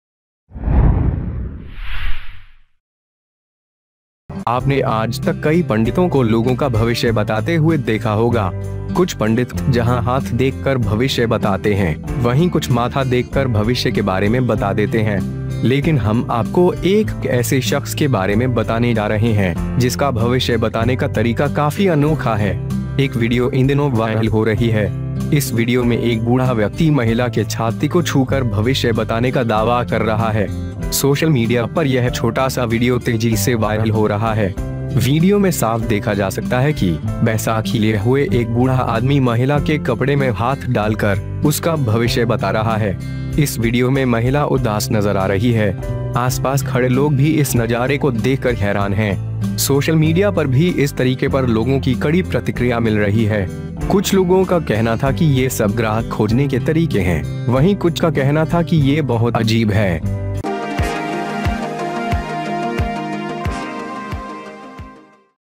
ये ज्योतिष महिलाओं की छाती छूकर बताता है उनका भविष्य, देखिये... || Astrology baba
A video is getting viral these days. In this video an old man is claiming to tell the future by touching the chest of the woman. This small video on social media is becoming increasingly viral. It can be seen clearly in the video that, an old man with crutches, holding hands in a woman's clothes, is telling his future.

In this video, the woman is sad. People around are also surprised to see this sight. There is also a strong response on social media in this manner. Some people said that these are the ways of finding all the customers, while some said that it is very strange.

Also Get The More Information From the Below Link

It's All ? About News ?? &Politics ?? .
So Keep ? Watching ? Us .